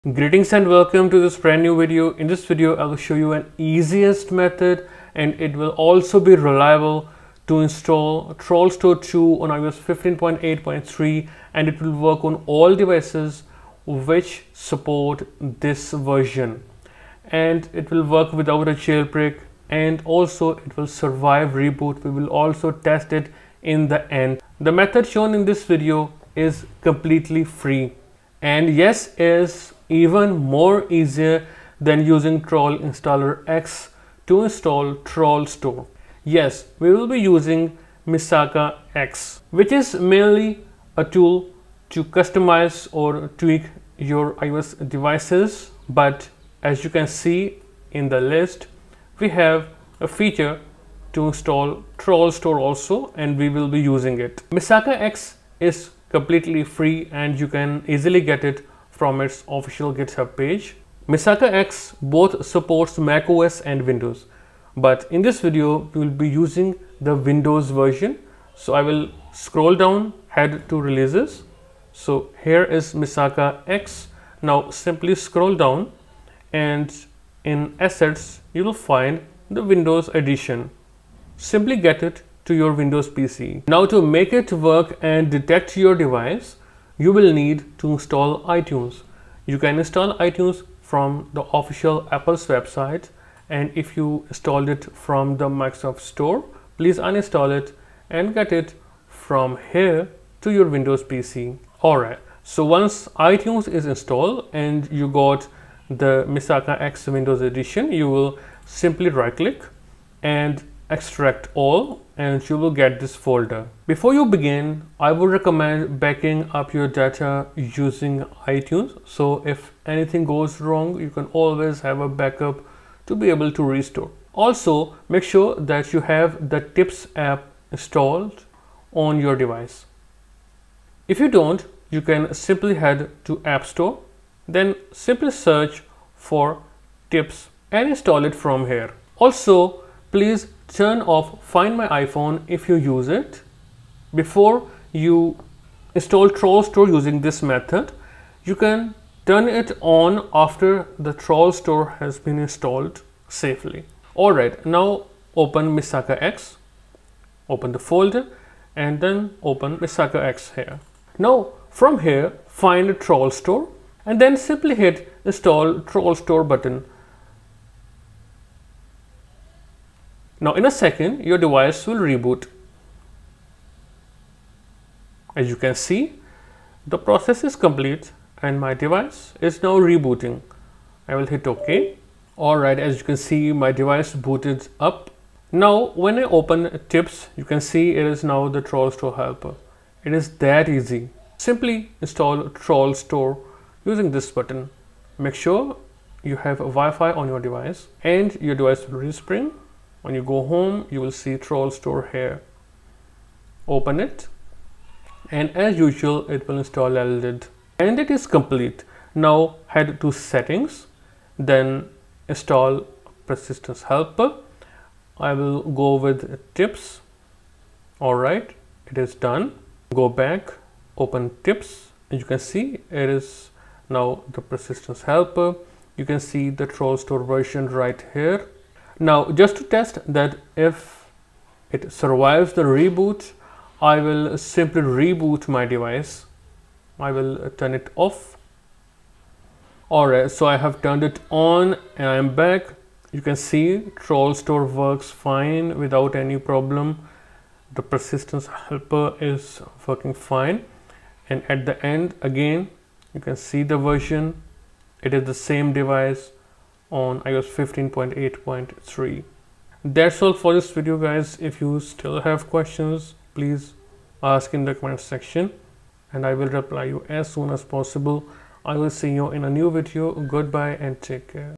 Get Trollstore iOS 15.8.3 with MisakaX Windows No Jailbreak | Survives Reboot | Trollstore 2 MisakaX
In this video, I'll walk you through the process of getting and installing Trollstore on iOS 15.8.3 with MisakaX on a Windows PC without jailbreak. This new method supports all iOS 15.8.3 devices, including iPhone 7/7+/6s/6s+/SE and iPads, and is both simpler and faster compared to TrollInstallerX, which was a lengthy process. The method shown is completely free, along with MisakaX and Trollstore 2.

Topics Covered:
Trollstore 2 MisakaX Guide
MisakaX Trollstore 15.8.3
iOS 15.8.3 Trollstore 2 Guide
Trollstore MisakaX 15.8.3